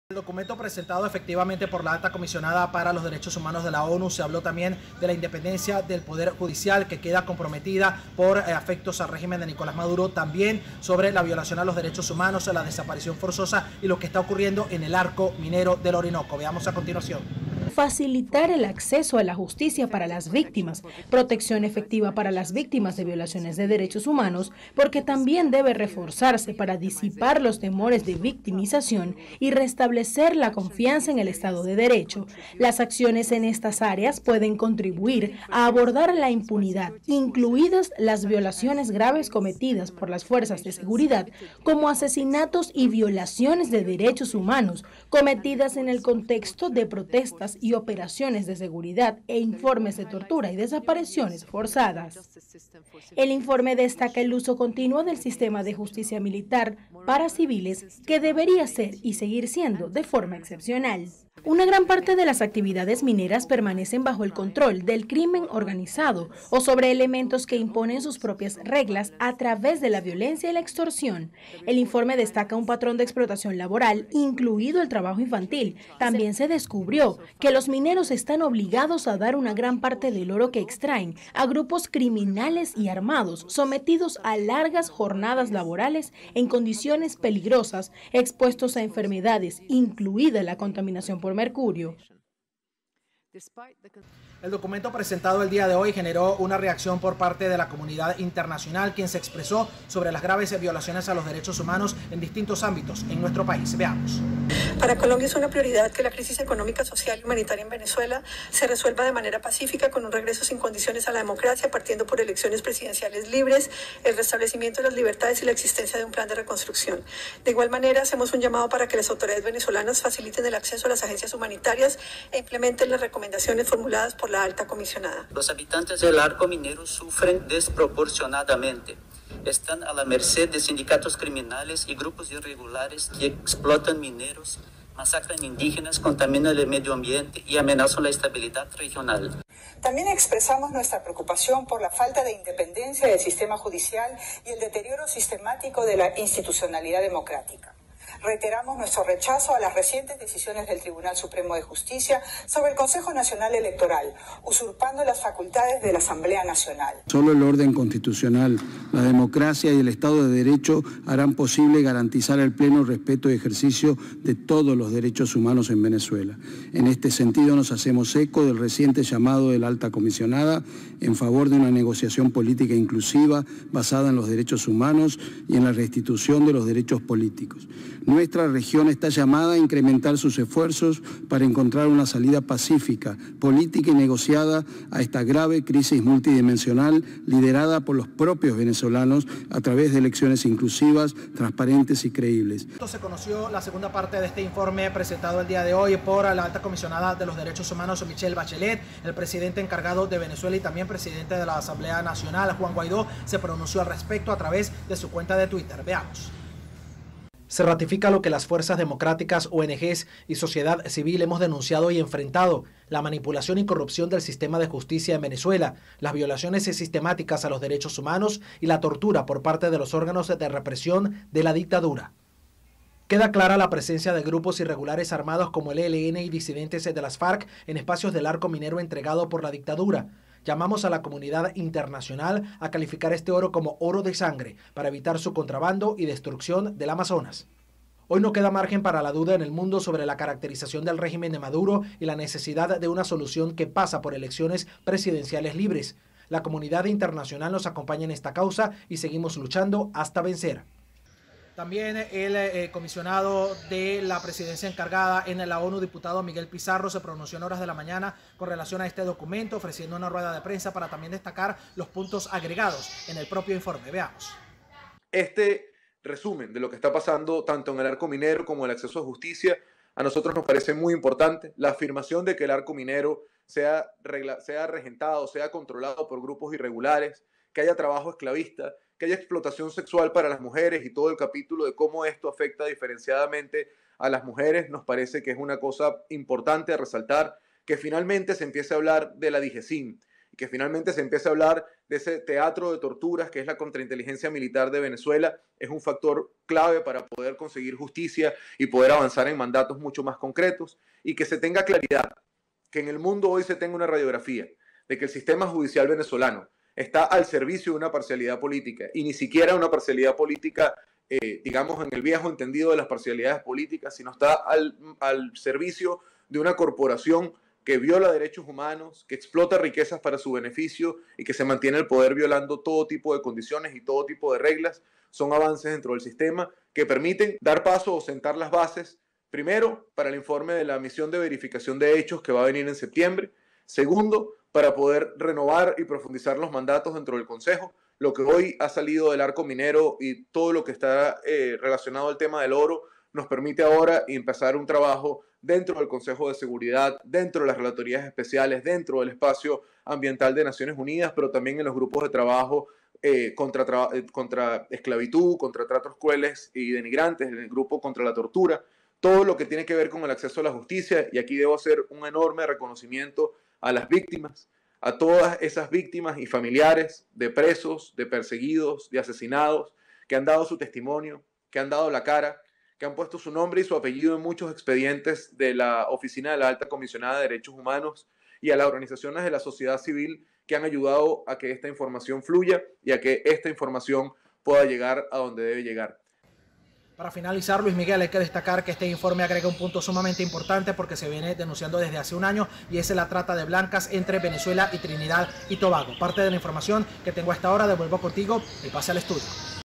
Informe de Bachelet visibilizó violaciones en el Arco Minero
La oficina de la Alta Comisionada para los Derechos Humanos de las Naciones Unidas, Michelle Bachelet, presentó este miércoles un informe que revela el control en el Arco Minero del Orinoco por parte del régimen venezolano y los sistemas de abusos y torturas cometidas en el país.
El comisionado para la Organización de Naciones Unidas, Miguel Pizarro, ofreció balance del informe sobre las violaciones de los derechos humanos en Venezuela, presentado este miércoles por la oficina de la Alta Comisionada Michelle Bachelet.
“Nos parece muy importante la afirmación de que el Arco Minero sea controlado por grupos irregulares, que hay trabajo esclavista y explotación sexual. Finalmente se empieza hablar de la Dgcim y el teatro de torturas”, precisó el comisionado.
De igual manera, consideró necesario que el mundo tenga claridad de lo que ocurre en Venezuela por parte del régimen que comete violaciones a los derechos humanos y explota riquezas para sus beneficios.